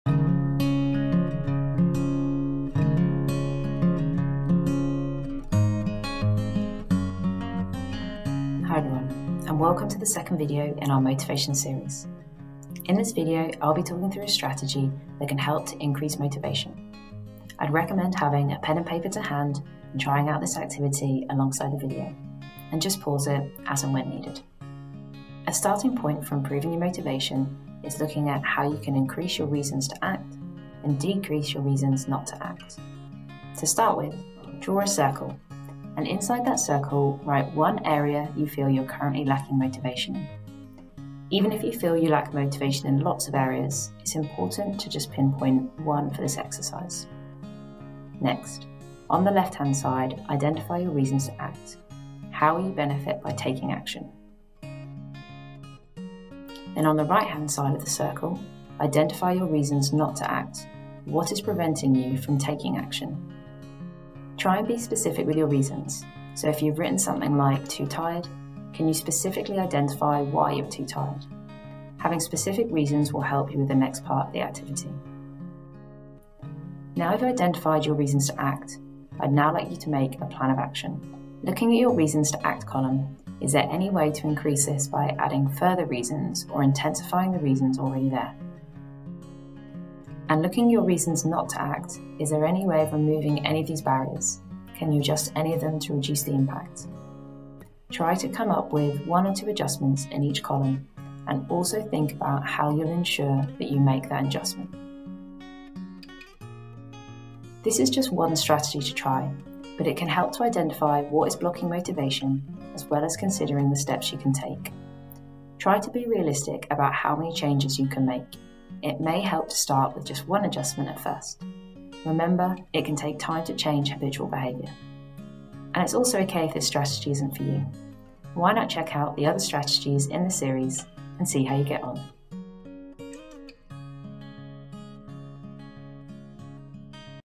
Motivation: Increasing action and decreasing inaction
This is the second video in our motivation series. In this video we talk through a strategy for improving motivation which involves increasing your reasons to act and decreasing your reasons not to act. Watch the video and try out the strategy alongside watching it.

Find out when our next wellbeing workshop called 'Motivation & Me' is taking place

Follow this link to find out more about Student Services and access support